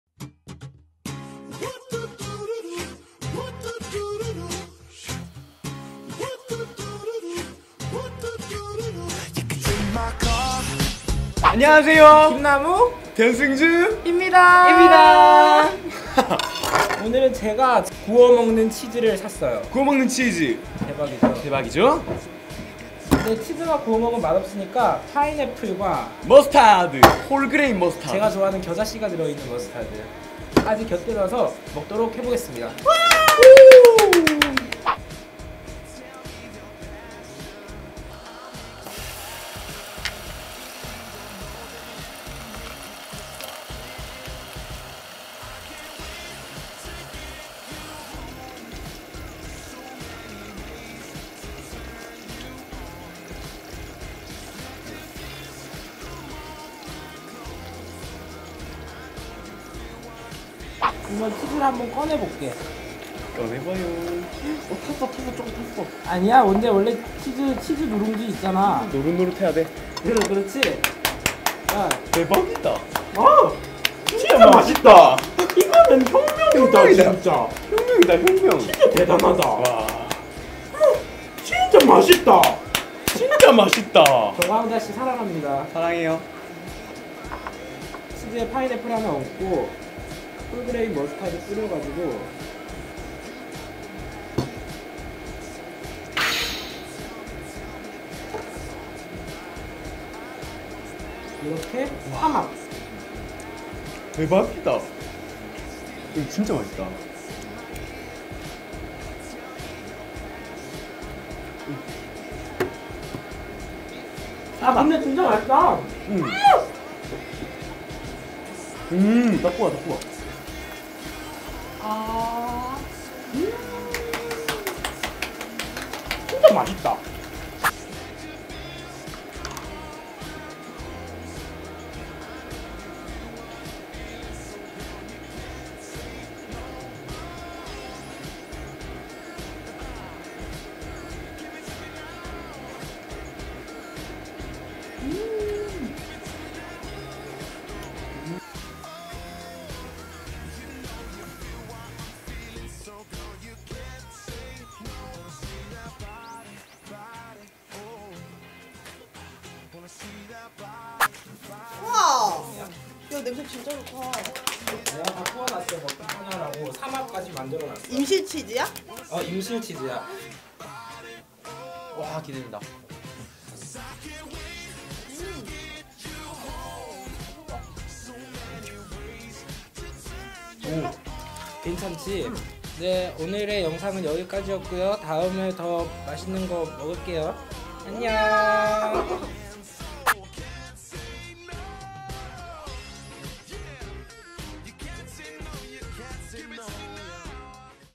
솔직한 리뷰 [직접 구워 먹는 치즈 먹방] 너무나도 느끼해서 즐거운 이야기 (feat. 공대생) - [김남욱]
우연히 티비에서 본 구워먹는치즈가 너무 먹고싶어서 바로 구매했어요 진짜 파인애플과 홀그레인머스타드의 조합이 개쩝니다 !

여러분들도 한번 사서 드셔보세요

재밌게보셨으면 좋아요과 구독도 잊지마시구욧!

남욱이의 욱기는일상 페이지